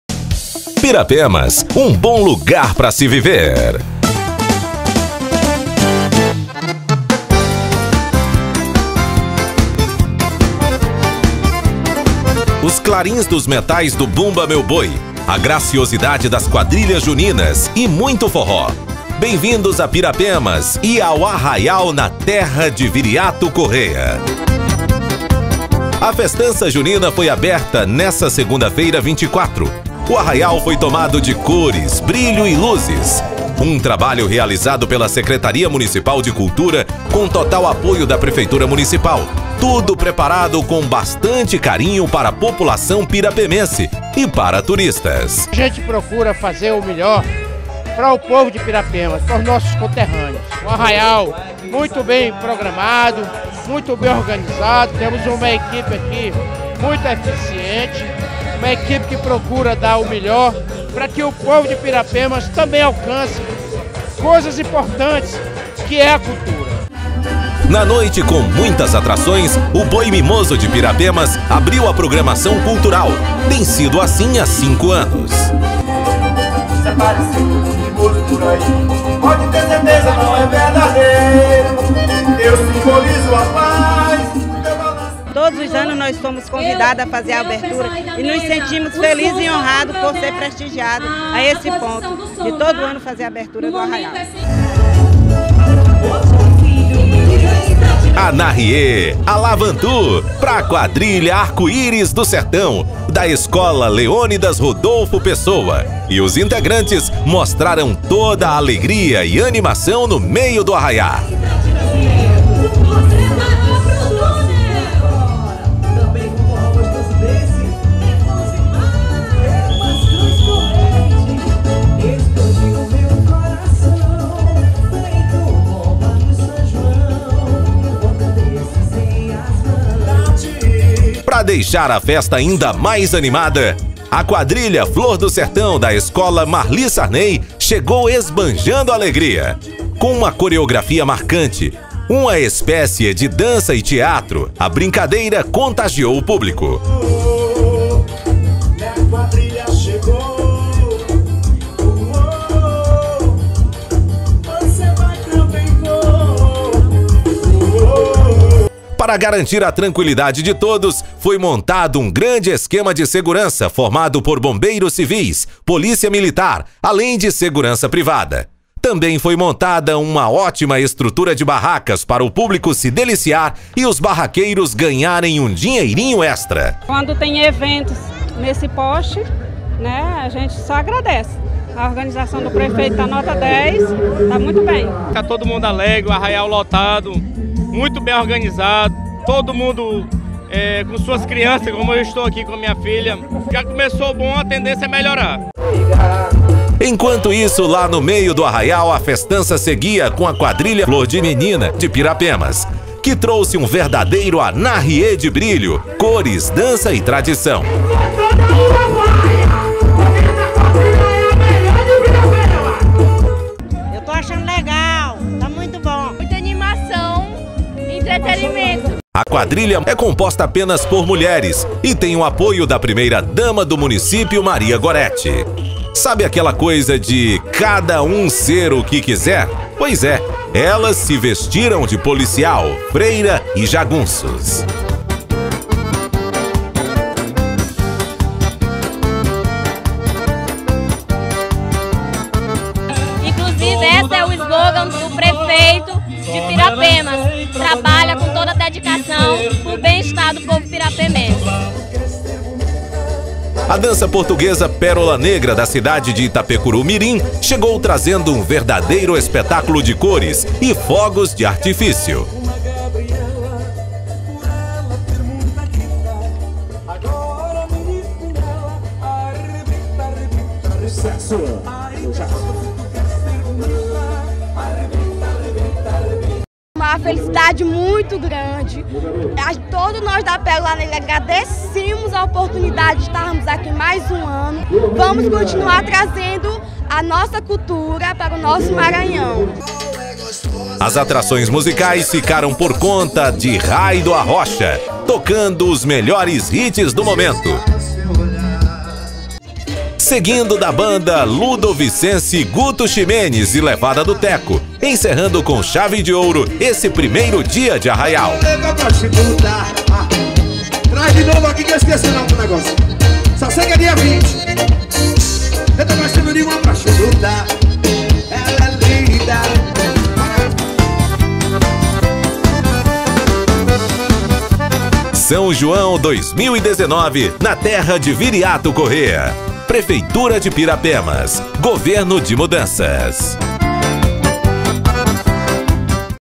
PIRAPEMAS - ABERTURA DO SÃO JOÃO NA TERRA DE VIRIATO CORREIA
Nesta segunda-feira (24), o arraial São João na terra de Viriato Correia recebeu várias brincadeiras, entre elas: Bumba Meu Boi, Quadrilhas e Dança Portuguesa. Encerrando a primeira noite com o show da banda Guto Ximenes.